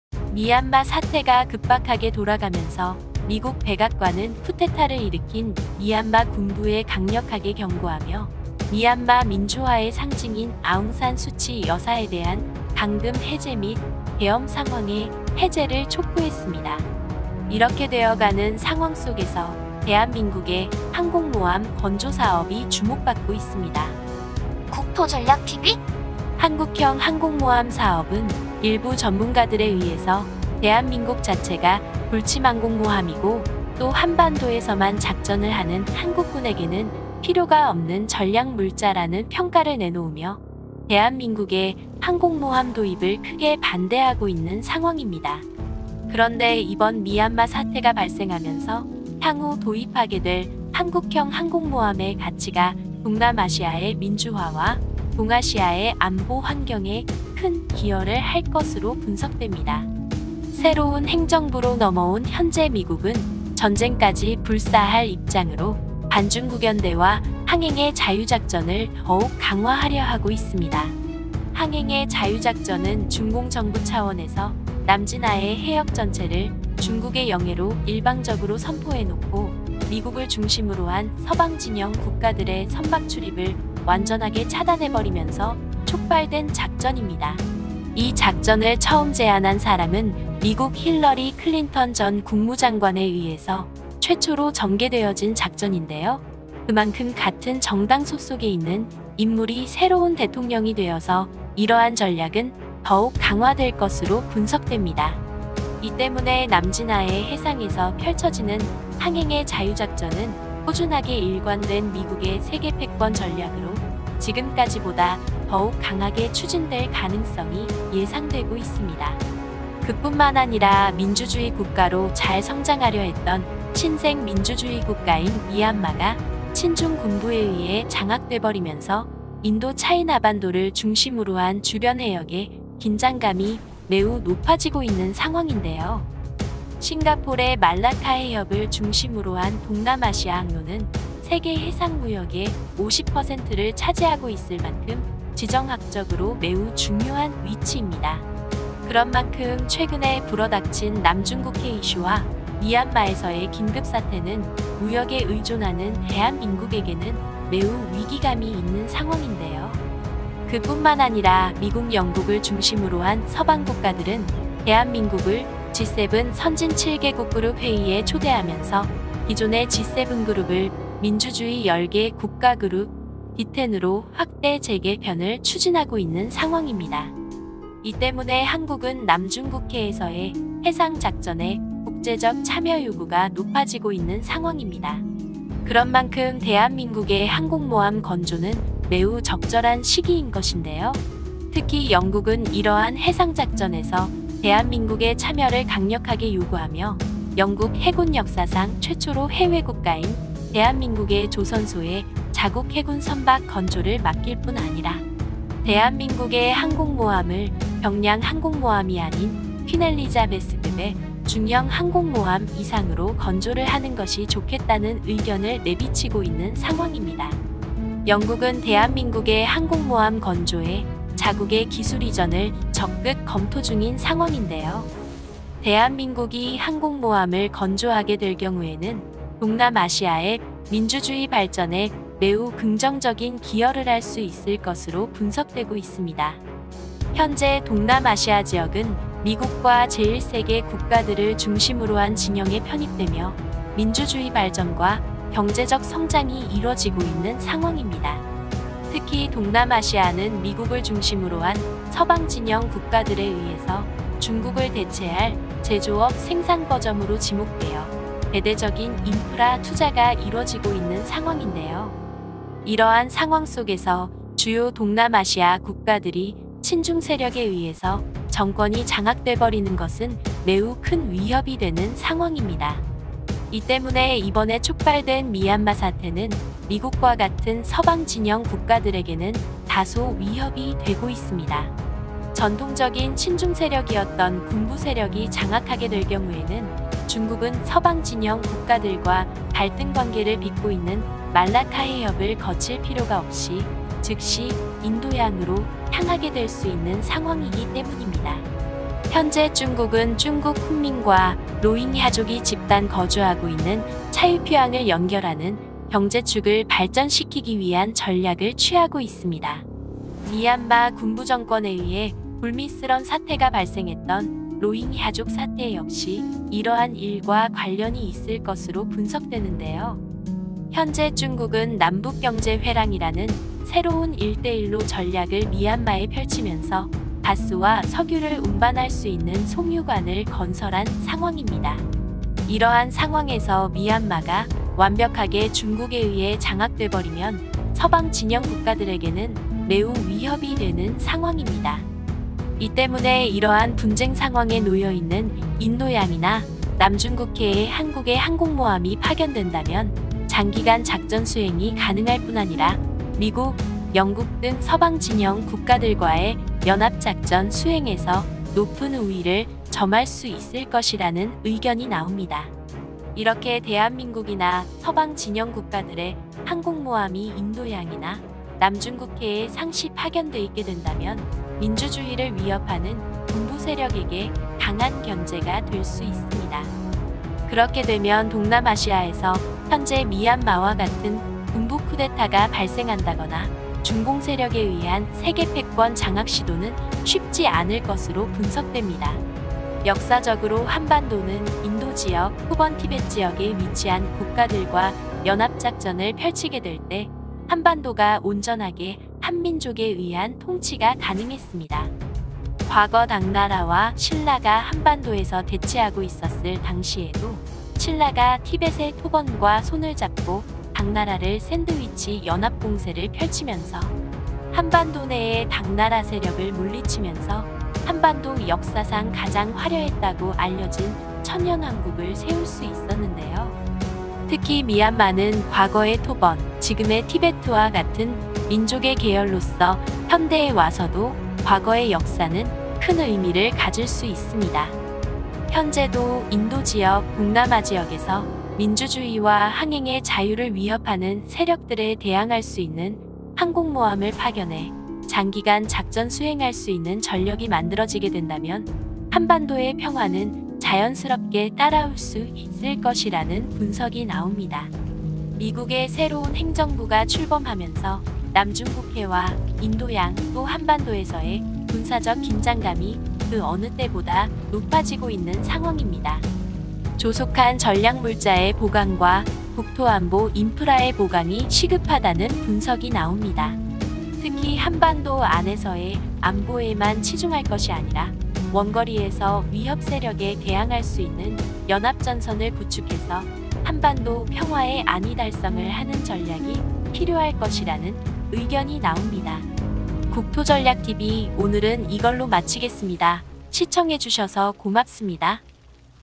미얀마가 위험에 처하자 한국의 항공모함이 크게 주목받은 이유 "빨리건조해서 파견하라"
미얀마 사태가 터지자 한국의 항공모함이 주목받는 이유는? * 영상의 내용 중에는 일부 사건에 대한 개인적 의견과 제안 혹은 개인적 의견이 반영된 해석이 담겨 있을 수 있음.